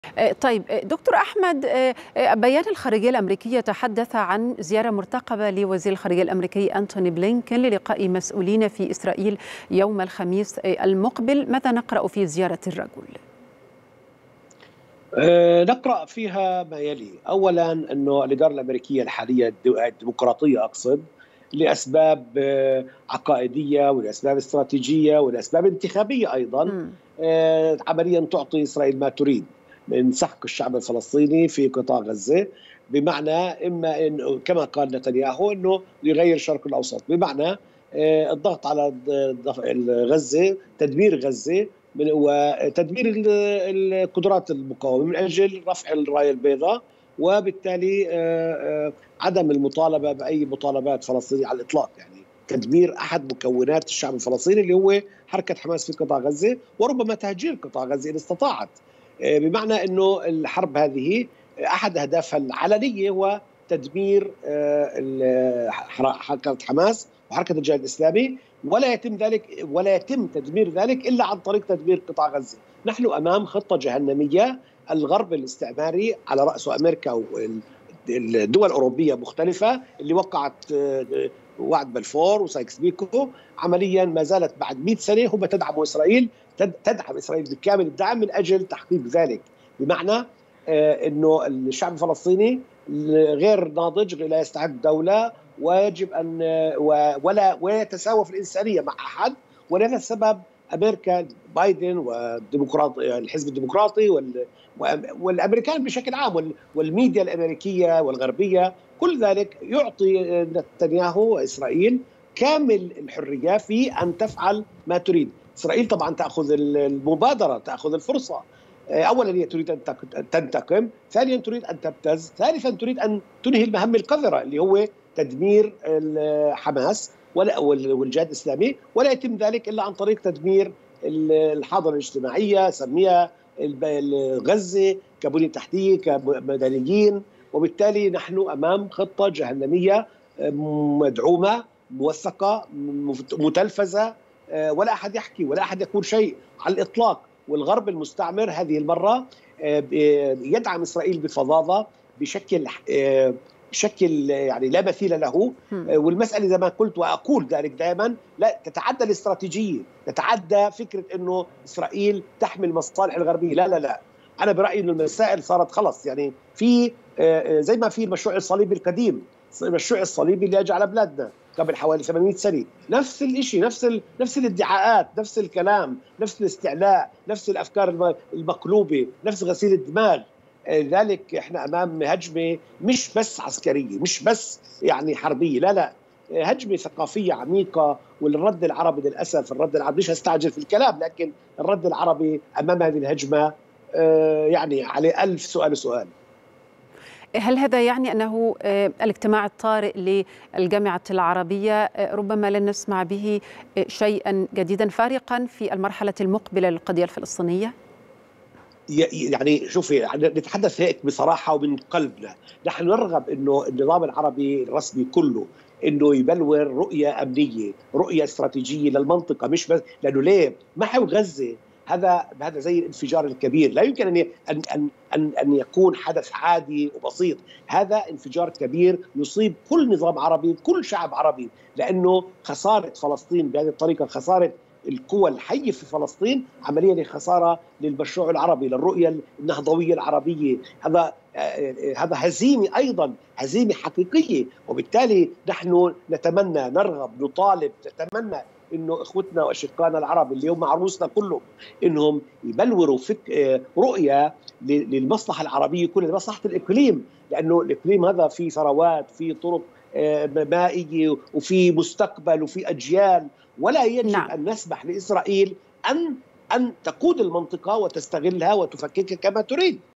د.أحمد رفيق عوض مدير مركز القدس للدراسات المستقبلية يوضح علاقة الغرب بالاشتباكات بين فلسطين وإسرائيل
10/10/2023